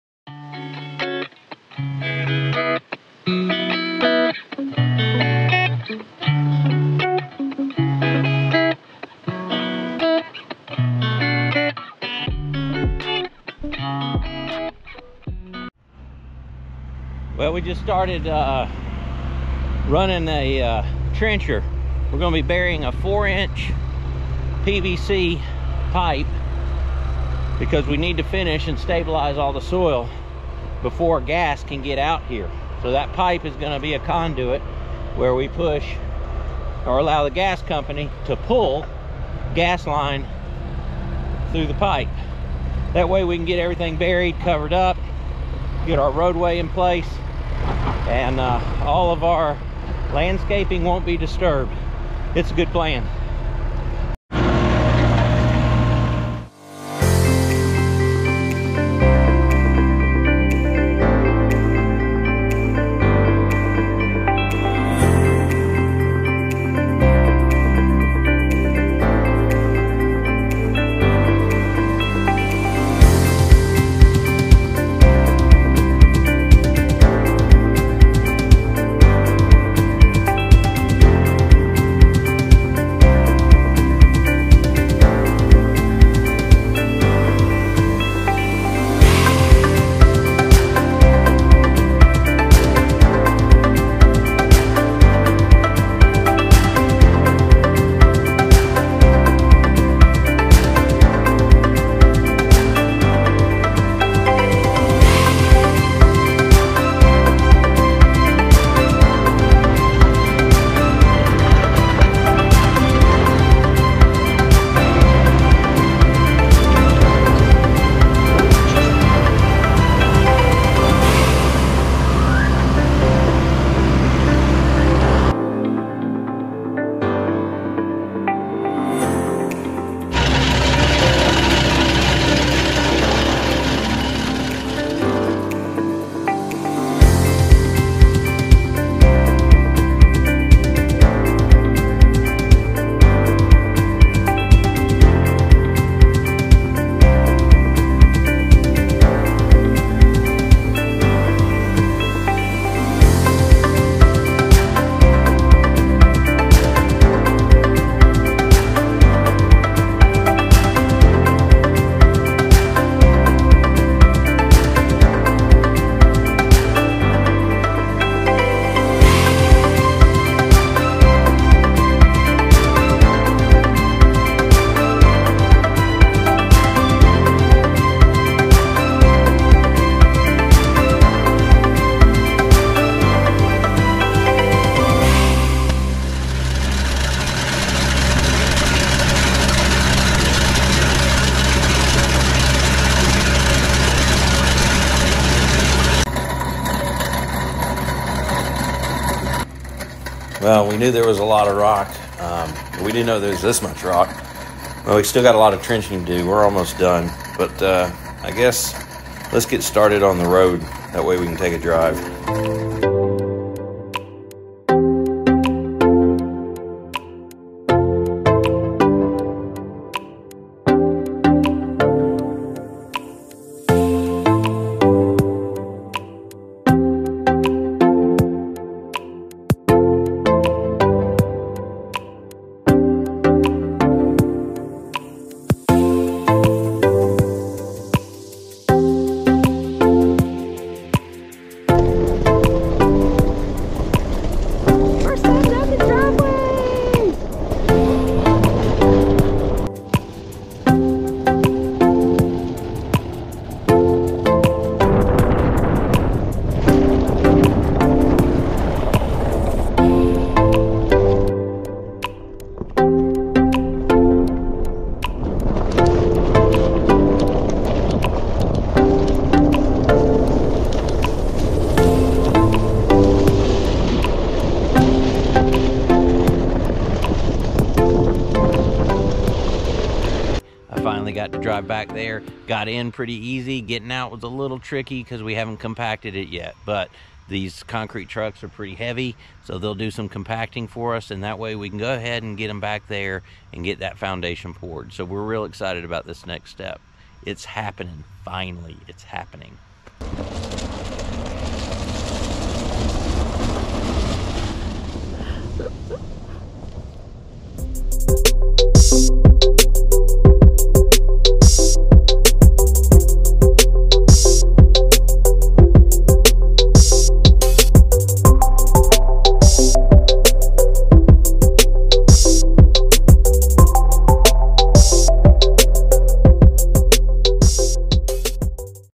Ep 12. Trenching and Gravel Drive
Trenching for water and gas line then gravel gets spread on the driveway.